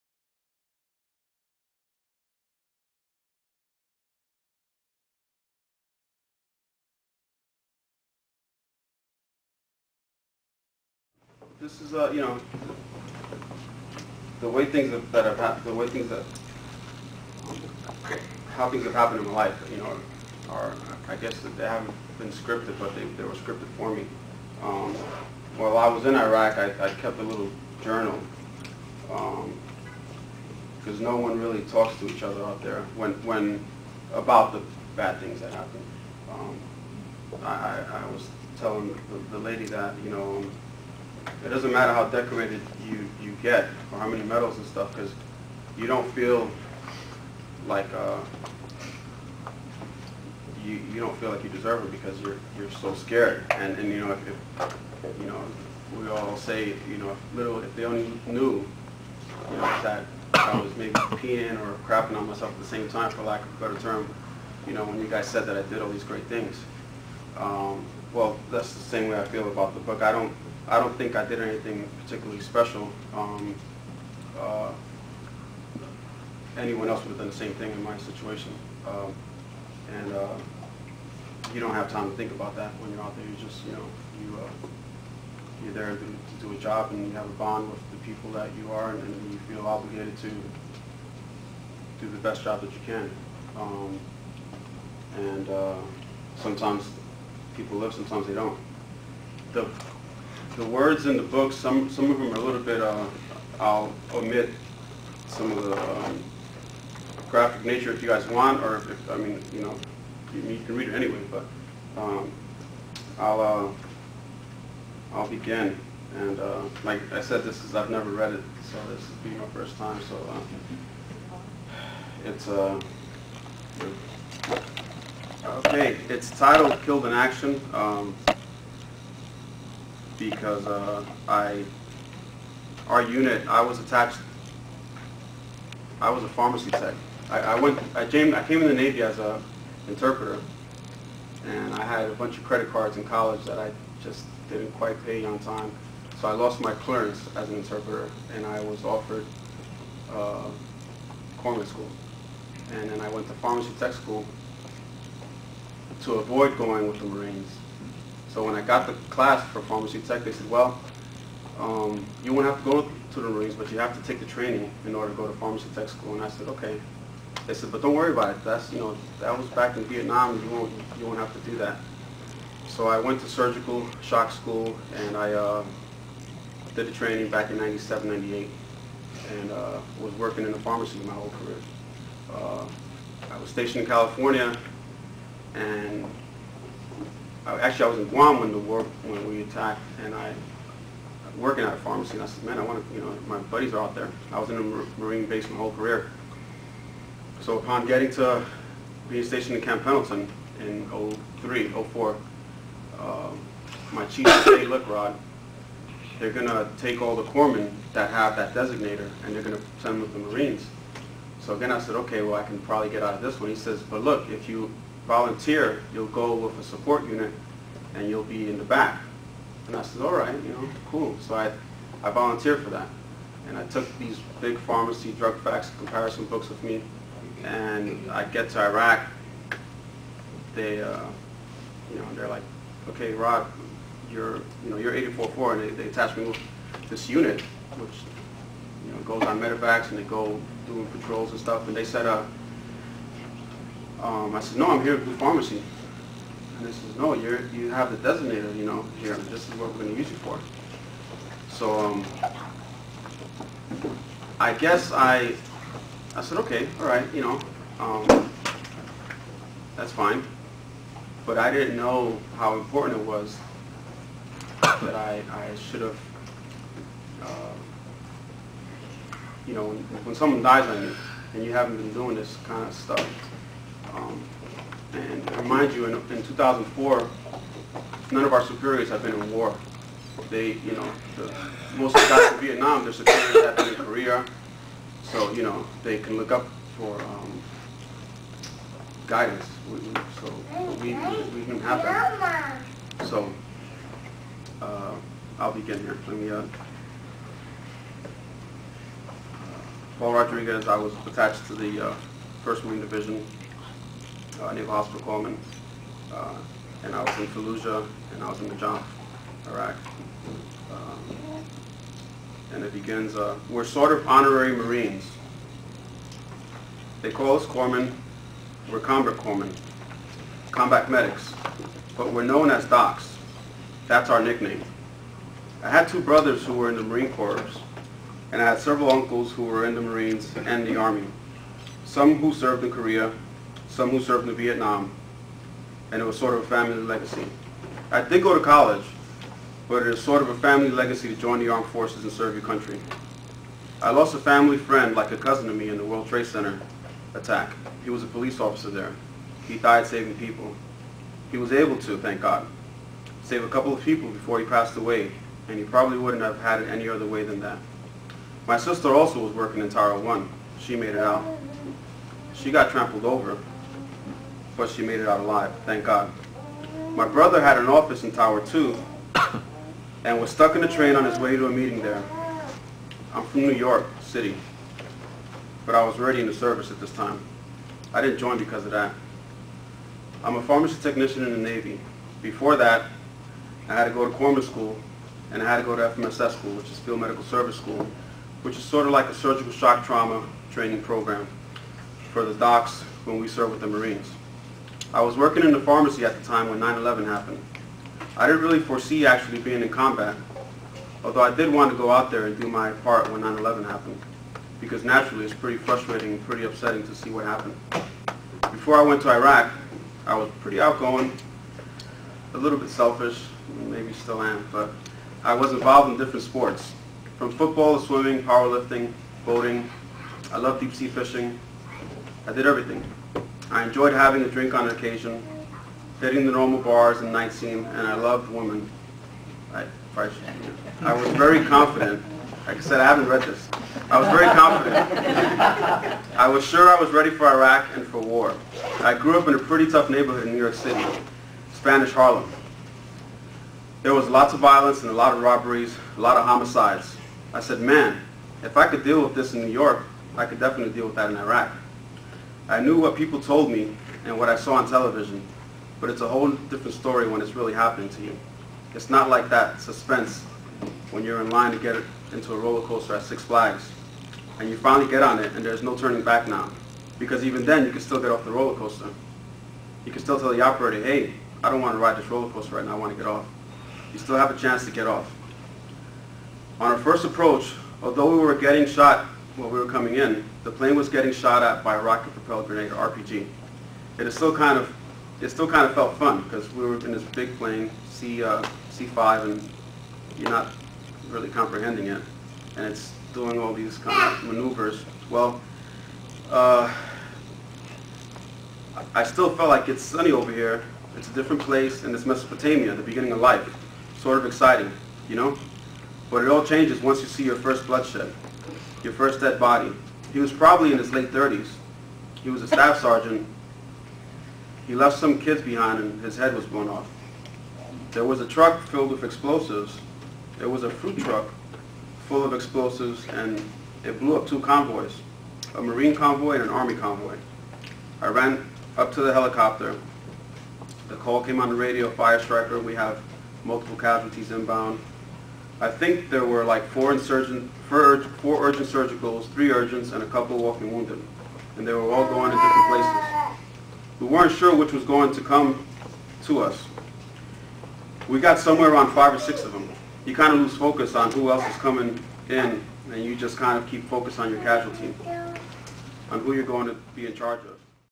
Talk with Veteran Paul Rodriguez Part 1
Veteran Paul Rodriguez visited with Kingwood Branch Library to share his experiences in Iraq as well as discuss "What Was Asked of Us" by Trish Wood.